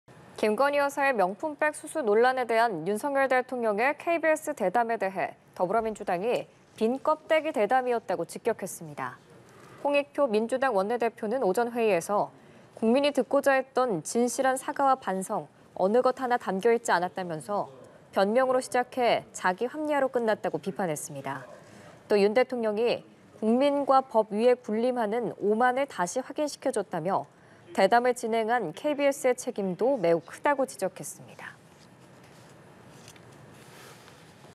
민주 "자기 합리화로 끝나…빈껍데기 대담" / 연합뉴스TV (YonhapnewsTV)
민주 "자기 합리화로 끝나…빈껍데기 대담"

김건희 여사의 명품백 수수 논란에 대한 윤석열 대통령의 KBS 대담에 대해 더불어민주당이 "빈껍데기 대담"이었다고 직격했습니다.

홍익표 민주당 원내대표는 오전 회의에서 "국민이 듣고자 했던 진실한 사과와 반성 어느 것 하나 담겨있지 않았다"면서 "변명으로 시작해 자기합리화로 끝났다"고 비판했습니다.

또 윤 대통령이 국민과 법 위에 군림하는 오만을 다시 확인시켜줬다며, 대담을 진행한 KBS의 책임도 매우 크다고 지적했습니다.